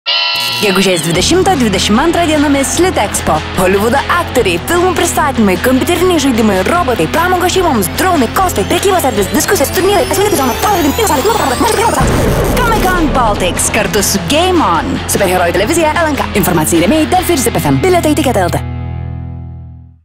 Comic Con Baltics 2022: Together with GameOn
Let's meet at Comic Con, on May 20-22, LITEXPO, Vilnius.
- Participate in Comic Con Baltics -
Comic Con Baltics the biggest popular culture event in the Baltic countries, attracting more than 20 thousand visitors and inviting people from famous Hollywood projects.